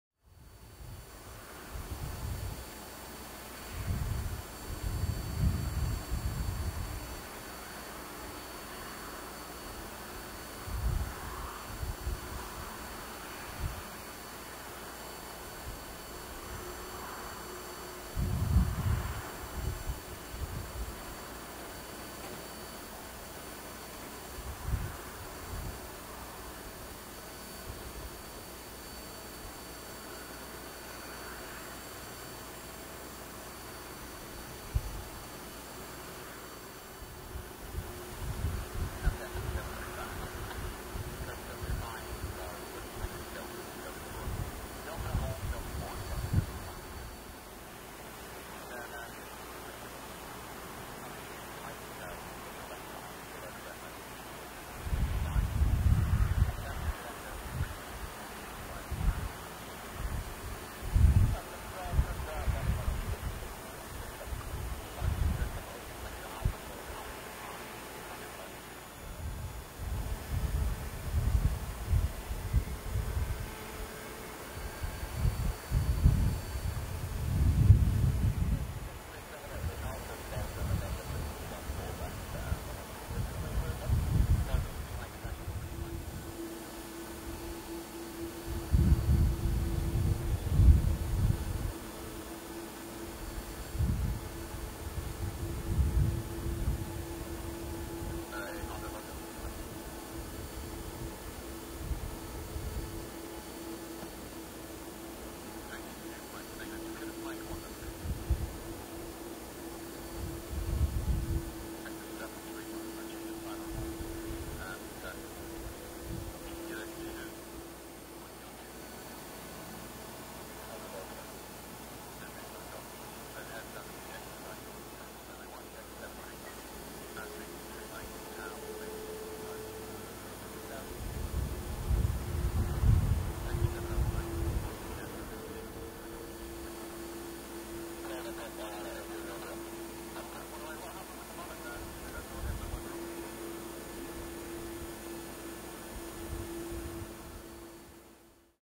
Thomson Airways | 787-800 Dreamliner | Pushback & Taxi | Manchester Airport | HD
Thomson Airways
Boeing 787-800 Dreamliner
Pushback, Start & Taxi
Manchester Airport